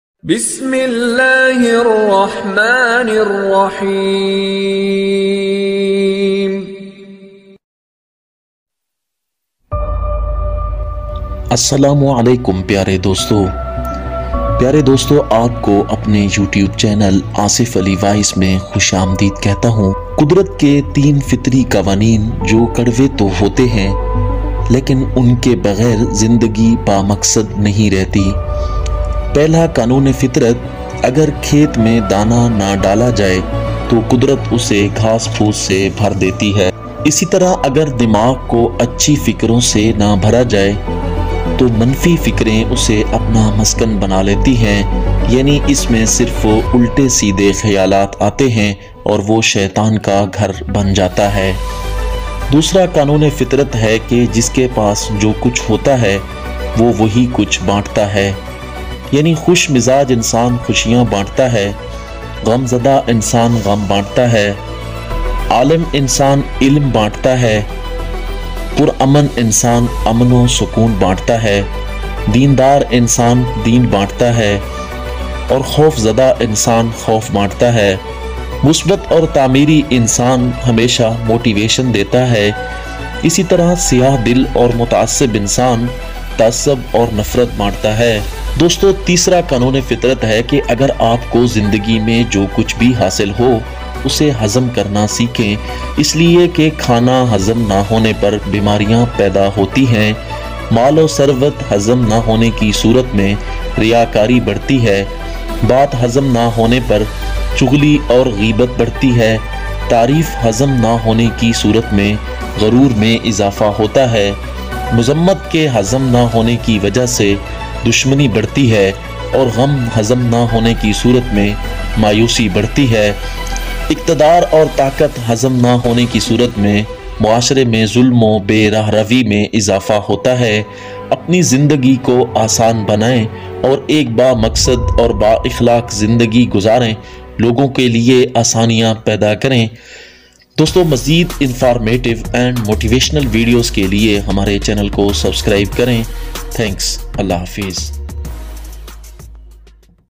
Three laws of nature that are bitter/Motivational stories in urdu& hindi/Asif ali voice
In this video, a motivational story about nature and the law of nature has been described in which three laws of nature have been described regarding the successful life of man.